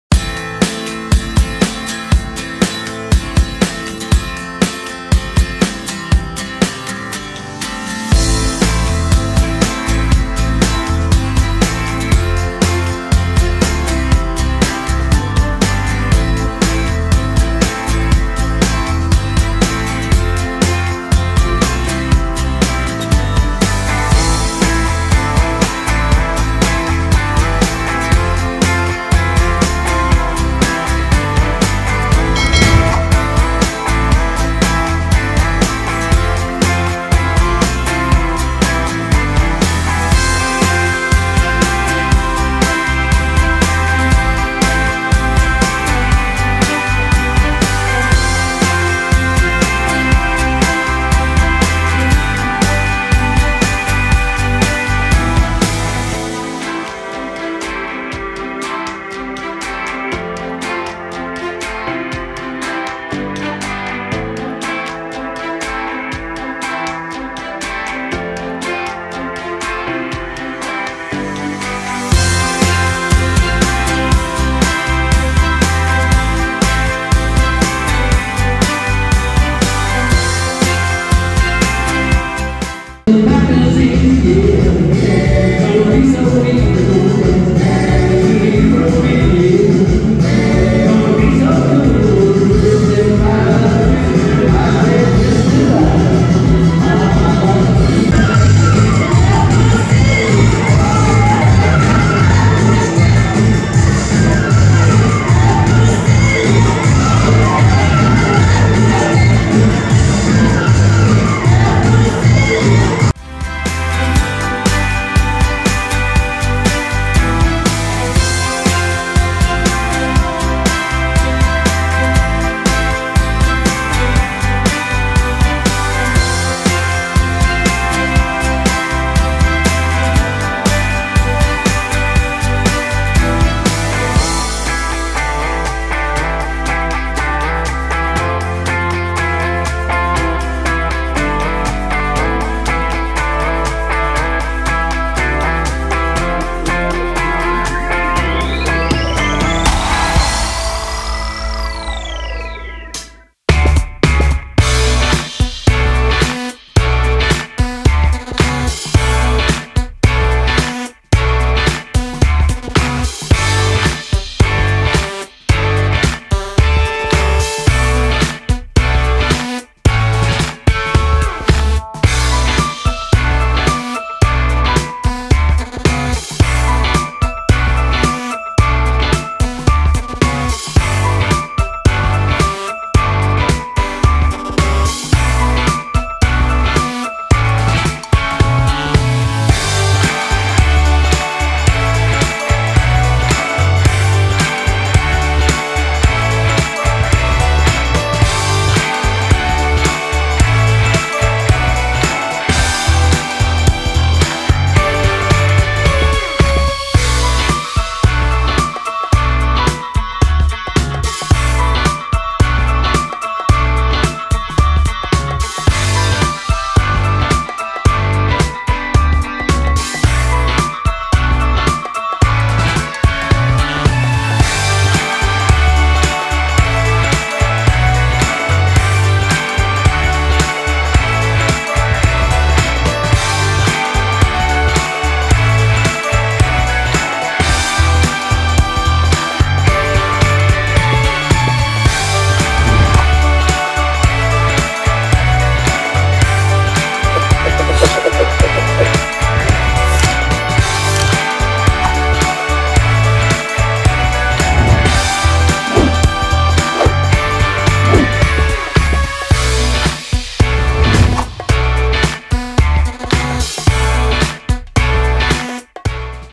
Accessible Music Festival | Gympie Muster 2024 | 24039
Hold on tight for a whirlwind of fun and unforgettable moments! Our time at Gympie Muster was packed with excitement and laughter. We kicked things off with a thrilling ride on the sugar cane train, explored the fascinating submarine museum, and had a blast at the Big Banana. The festival was a highlight, where we line danced to country music and soaked up the festive vibes. Cruising around the beautiful Noosa River was pure bliss, and sharing delicious food with new friends made the experience even more special. Join us as we relive these amazing memories and celebrate the joy of travel and friendship!